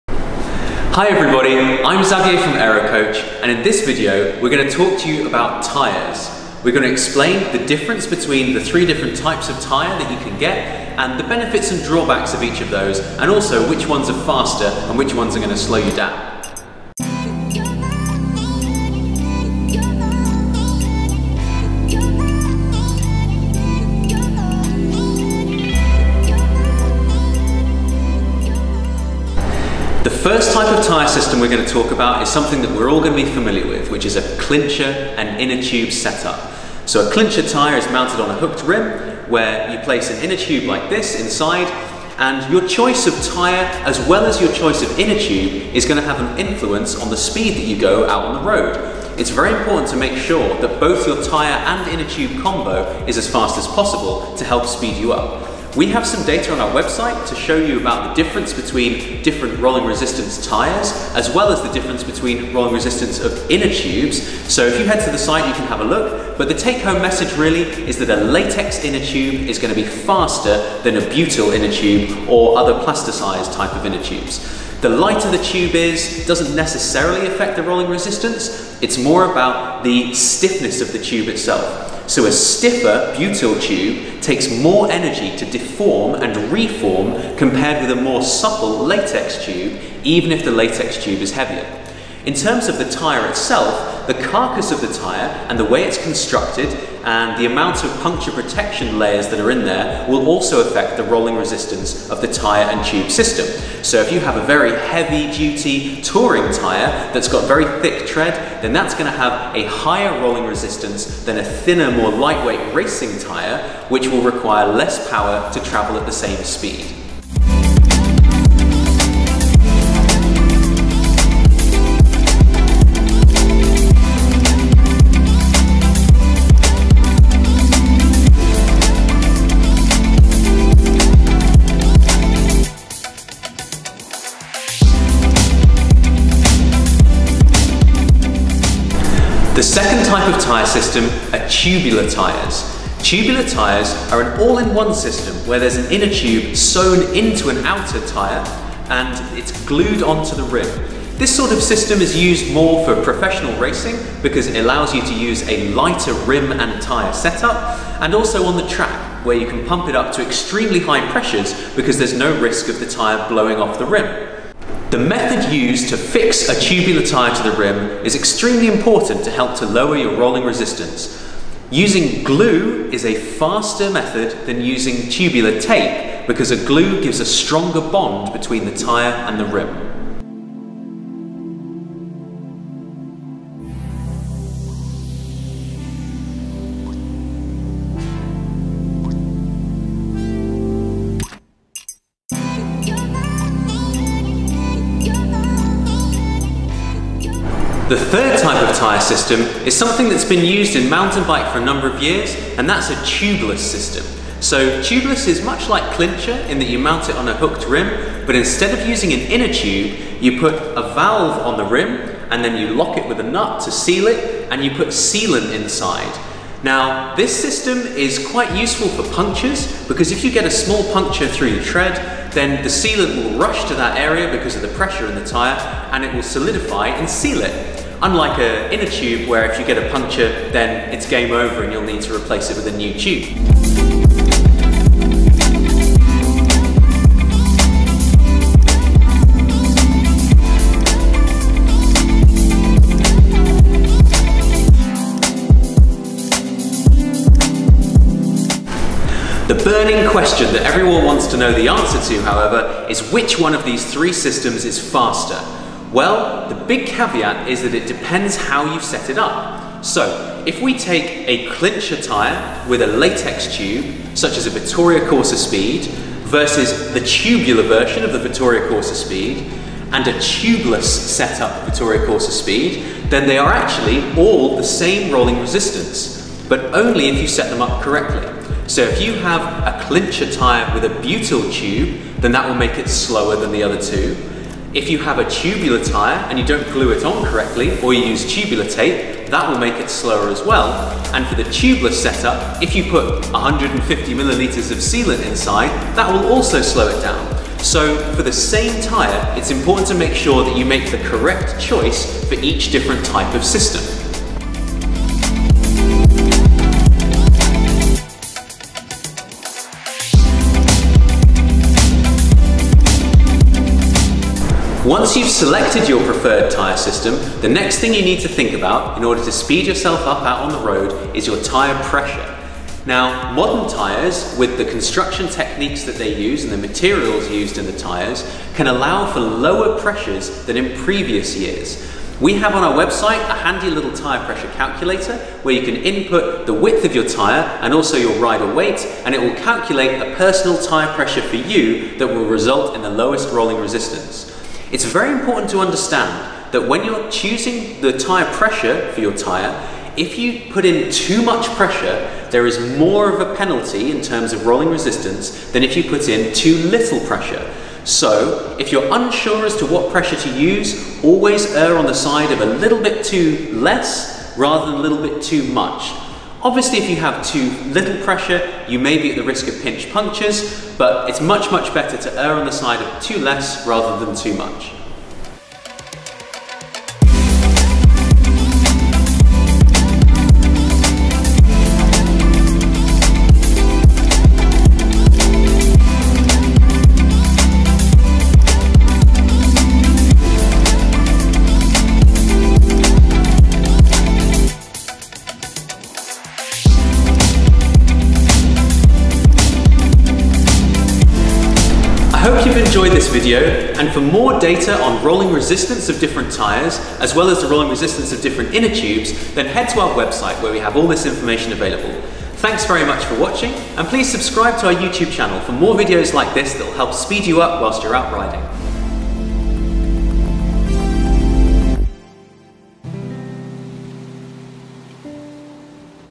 CHOOSING THE BEST TYRES - clincher vs tubular vs tubeless!
Choosing the right tyres is important to help speed you up out on the road - in this video Dr Xavier Disley explains how different tyre systems work and which is the best, as well as tips for each system.